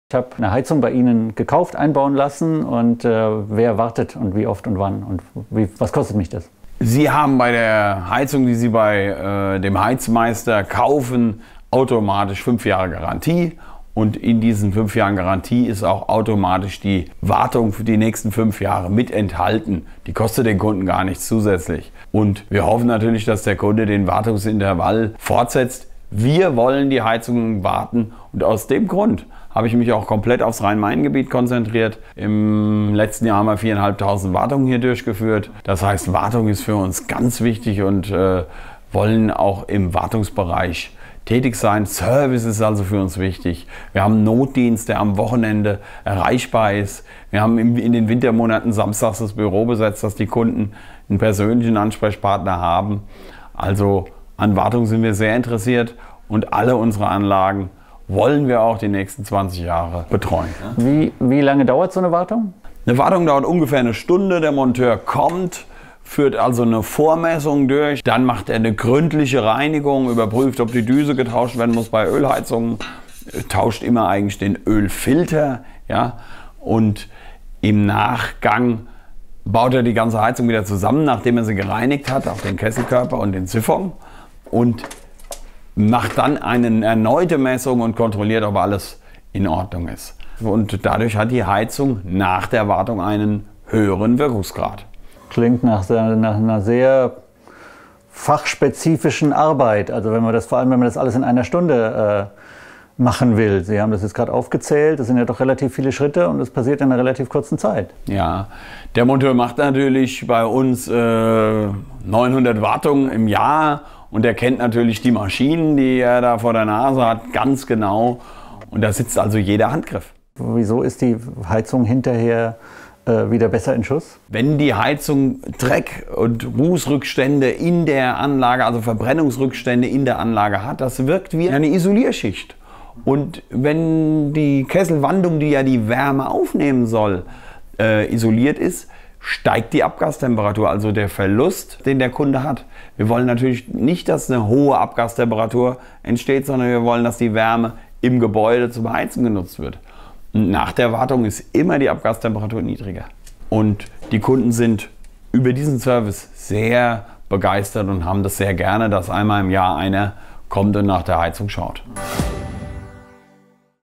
Wartungs-Talk
Viele Informationen rund um die Heizungswartung vom Heizmeister.